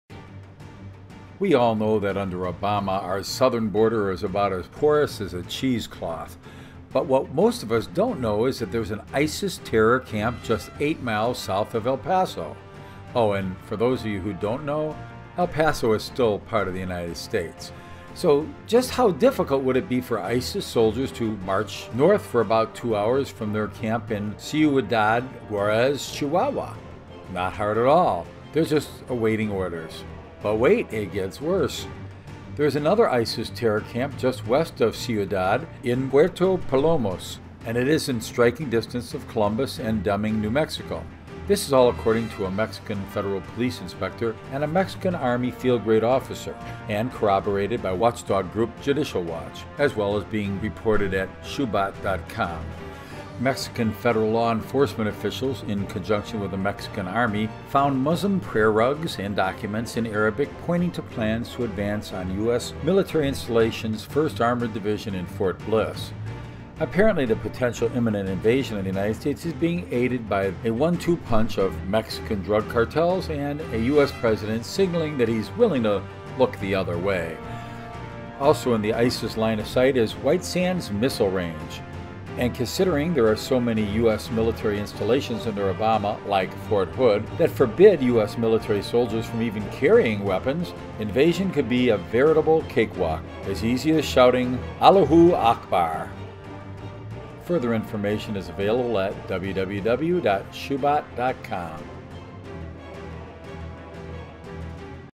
Terrorist camp 8 miles from El Paso, TEXAS!
We all know that under Obama, our Southern Border is about a porous as a cheese cloth.

But what most don't know is that there is now an Terrorist Camp just 8 miles south of El Paso.

Oh, and for those who don't know, El Paso is part of The United States,  for now at least.

So just how difficult would it be for Terrorists to march north for about 2 hours from their camp in Ciudad Juarez, Chihuahua? Not hard at all. They're just awaiting orders.

But wait, it gets worse. There is another terror cell just west of the Ciudad Juarez camp, in Puerto Palomas that is in striking distance of Columbus and Deming, New Mexico.

This is all according to a Mexican Federal Police Inspector and a Mexican Army field grade officer, and corroborated by watchdog group, Judicial Watch, as well as being reported at shoebat.com.

Mexican federal law enforcement officials in conjunction with the Mexican Army found Muslim prayer rugs and documents in Arabic pointing to plans to advance on U.S. military installation's 1st Armored Division in Fort Bliss.

Apparently the potentially imminent  invasion of the United States is being aided by the one-two punch of Mexican drug cartels and a U.S. President signaling that he's willing to look the other way.

Also in the ISIS line of sight is the White Sands Missile Range. And considering there are so many U.S.  military installations under Obama, like Fort Hood, forbid U.S. military soldiers from carrying weapons, invasion could be a veritable cake walk: As easy as shouting: "Allahu Akbar!"